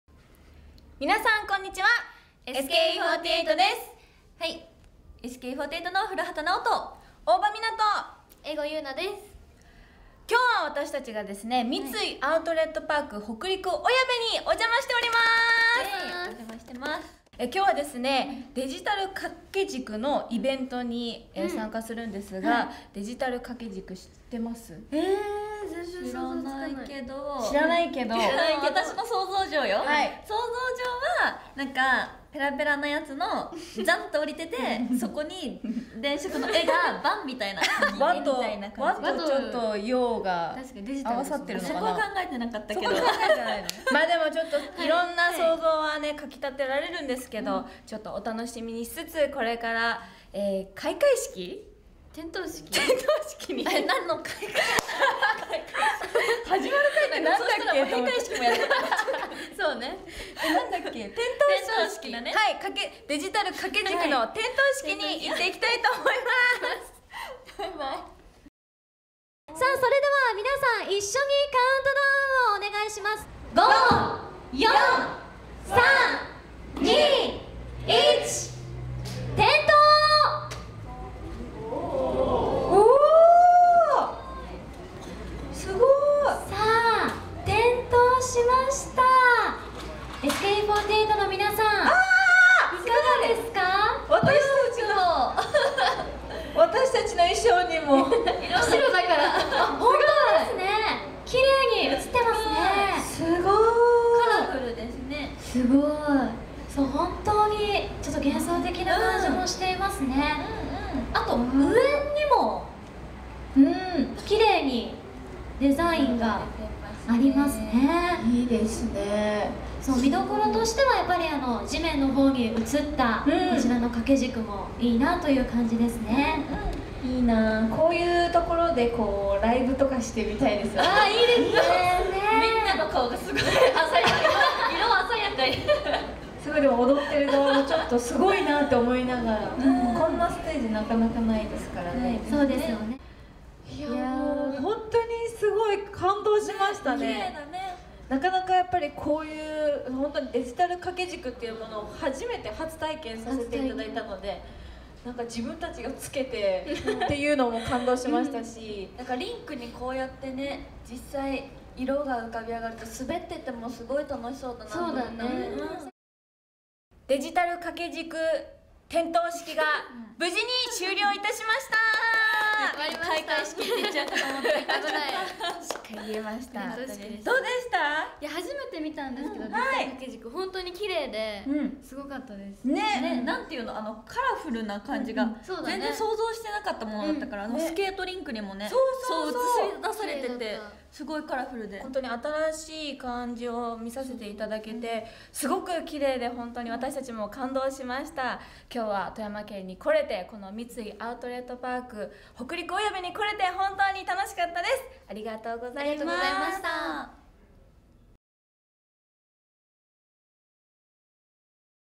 「デジタル掛け軸」点灯セレモニーにSKE48の3人が登場！
SKE48の江籠裕奈、大場美奈、古畑奈和が12/23(祝)に開催した「デジタル掛け軸～光のアートウォール～Xmas & New Year」点灯セレモニーに登場。「デジタル掛け軸」初体験の3人の反応は!?